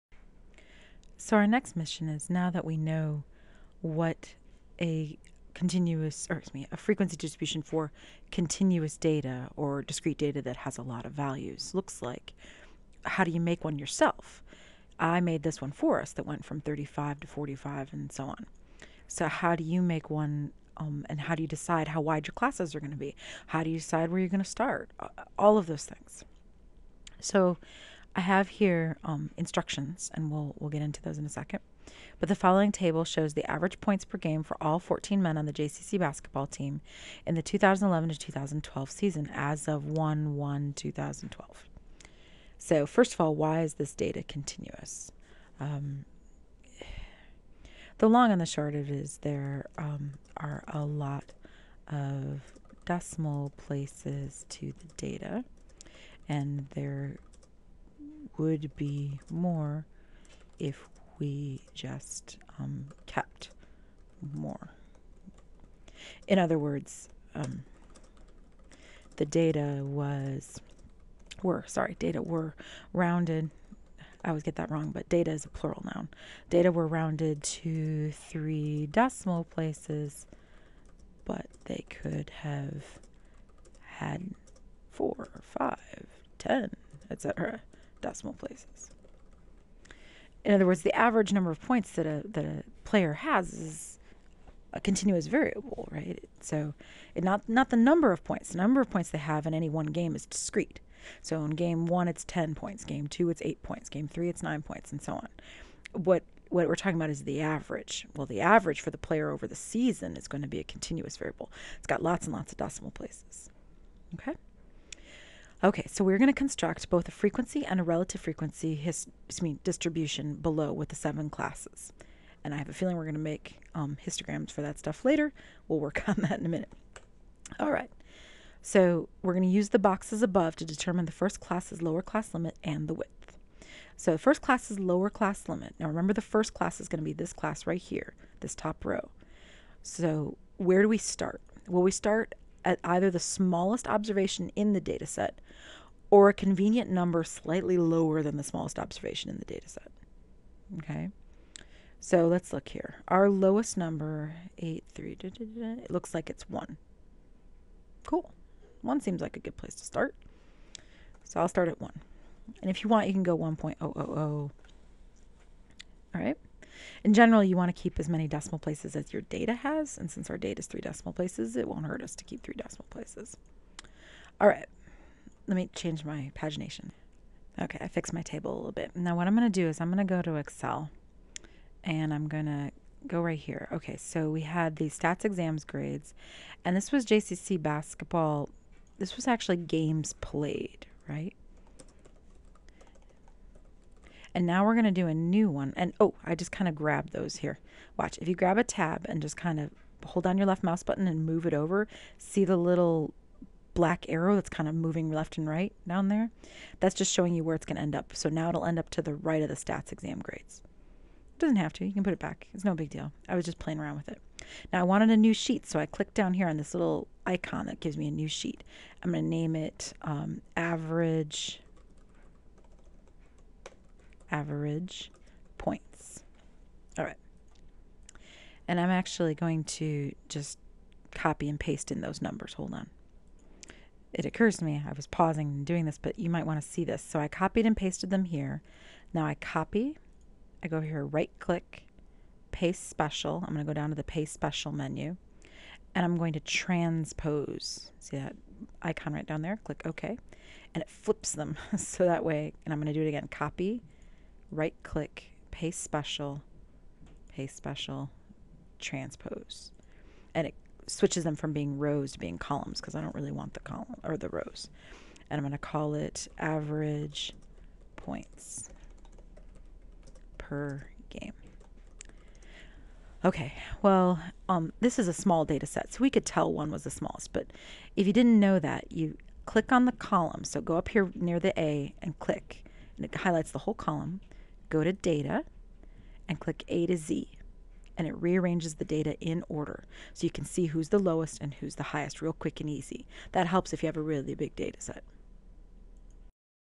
2.2 Lecture - Part 8 of 19 - Math 133
Covers: Constructing a frequency distribution of continuous data using Excel; Copying and "Paste Special" to transpose data, sorting data  (2.2 Notes, page 10)

Primarily meant for Math 133, PSY 144, CIS 205 students at Jackson Community College, MI.